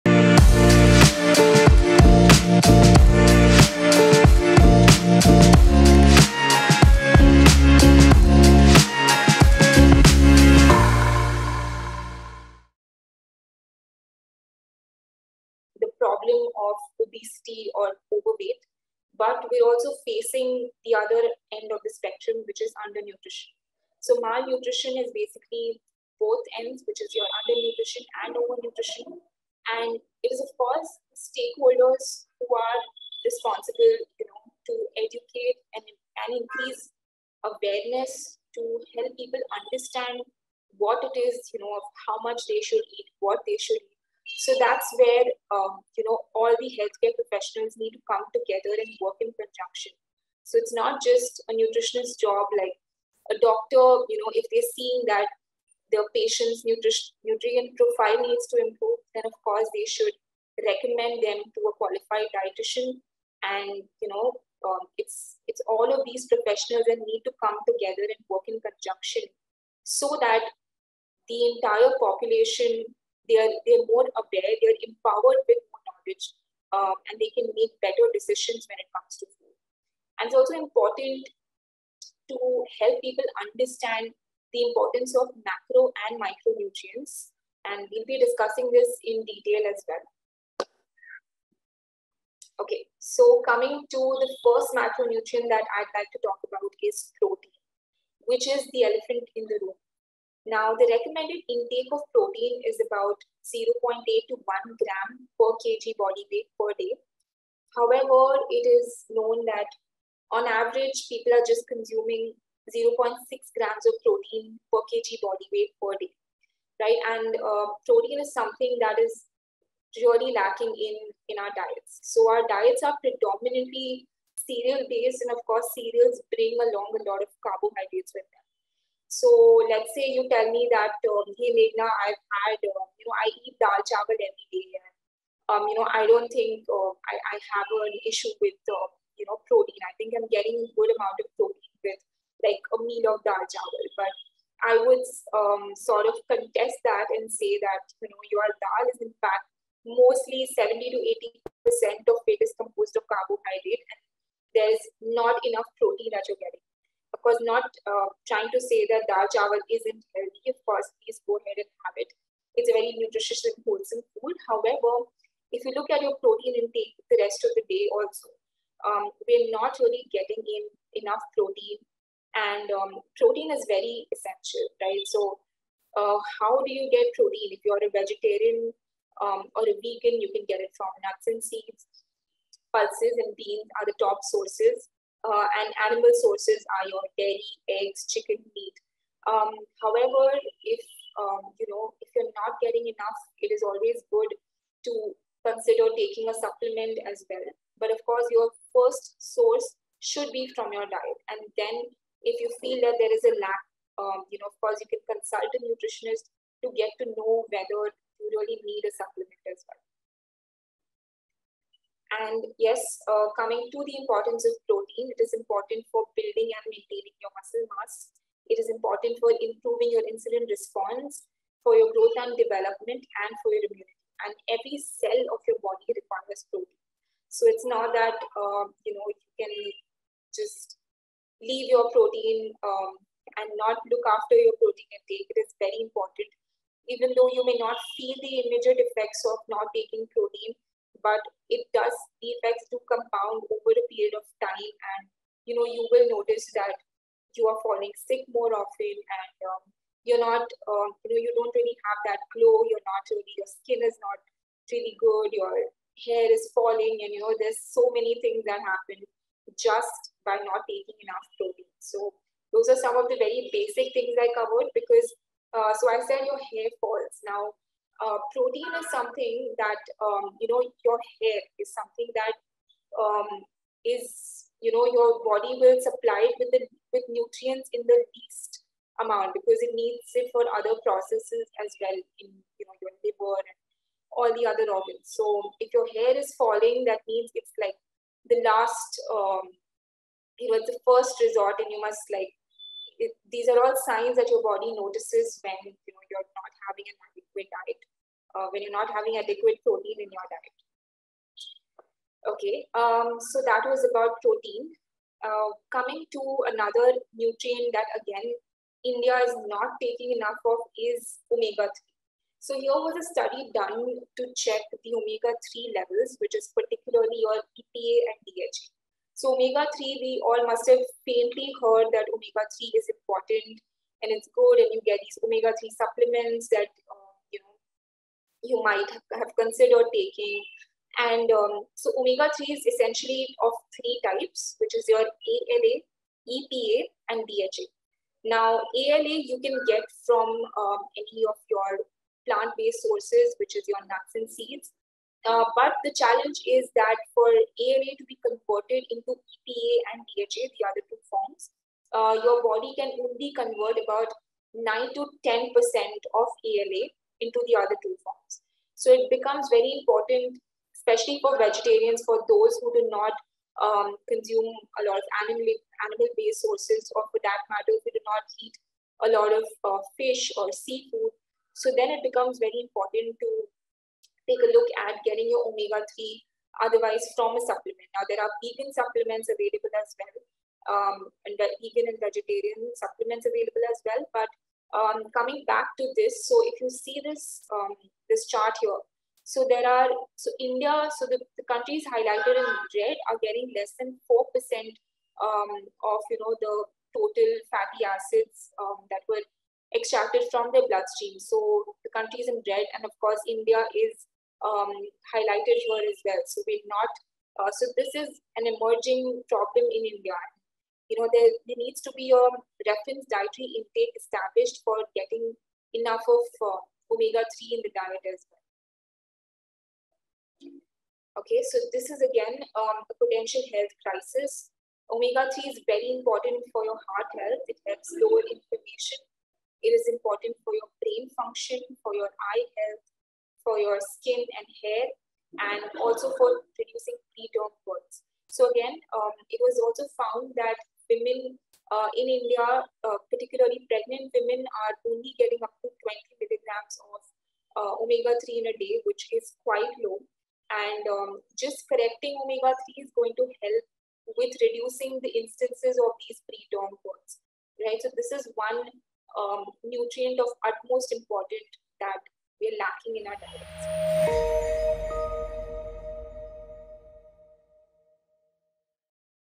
Preventive Measures of Malnutrition - Case Review
Join Nt. Meghna Tiwatia as she delves into India's dual challenge of malnutrition and overnutrition. Explore the essential steps and key stakeholders involved in improving nutrition status. Gain insights into portion control and the significance of macro and micronutrients.

In this enlightening video, Nt. Meghna Tiwatia emphasizes the importance of protein in building and maintaining muscle mass, enhancing insulin response, and supporting growth, development, and immunity. She highlights the crucial role of Omega-3 fatty acids in promoting brain function, eye health, skin and hair vitality, heart health, and reducing inflammation.